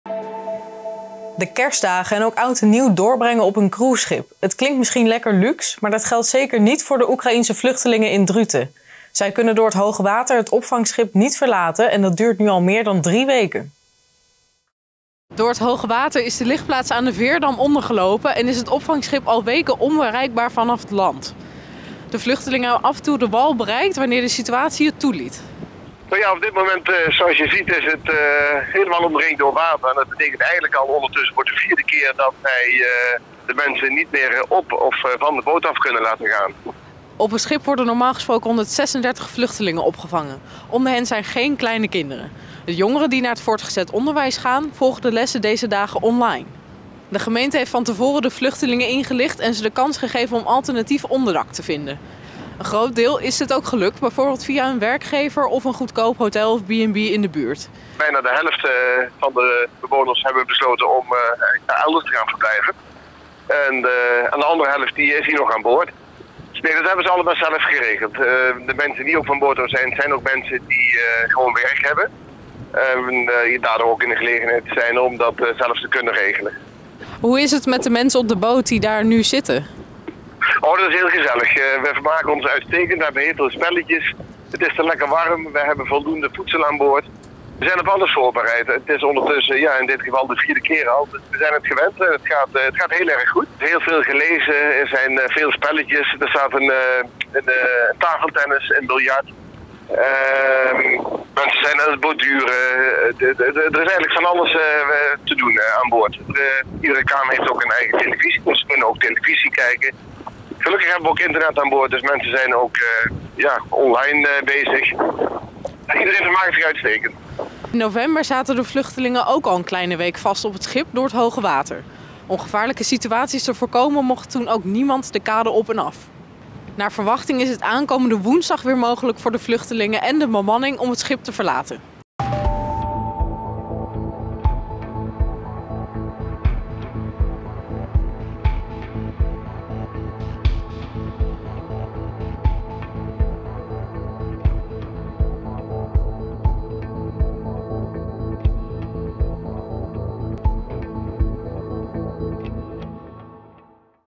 Vastzitten op cruiseschip door hoge water || RN7 REGIONIEUWS
De kerstdagen en ook oud en nieuw doorbrengen op een cruiseschip. Het klinkt misschien lekker luxe, maar dat geldt niet voor de Oekraïense vluchtelingen in Druten. Zij kunnen door het hoge water het opvangschip niet verlaten en dat duurt nu al meer dan drie weken.